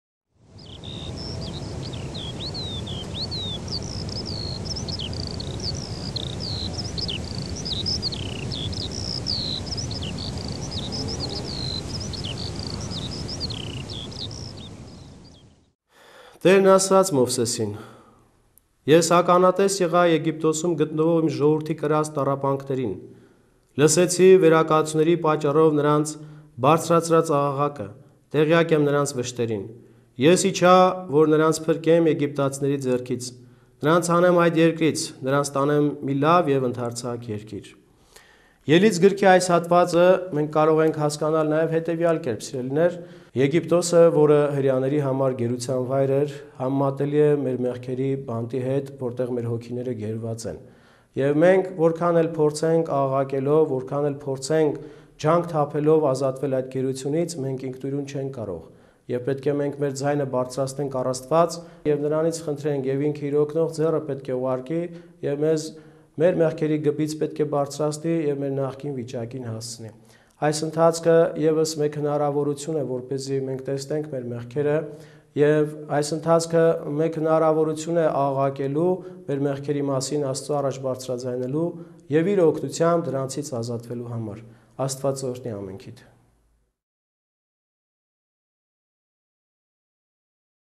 Մարտի 11
2020թ. մարտի 11-ի օրվա խորհուրդը (11 Դշ. ԱՁ. ԺԷ օր Մեծի Պահոց: Ելք. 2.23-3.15: Յովէլ 2.21-32:) մեկնում է Տ. Զարեհ քահանա Աշուրյանը: